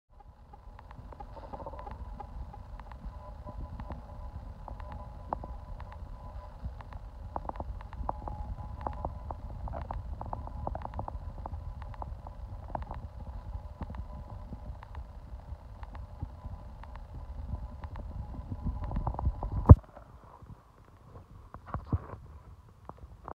Arctic Char Fishing in the foothills of the Torngat Mountains - Aug 17, 2019.
Incrediable footage of Arctic Char prior to spawning.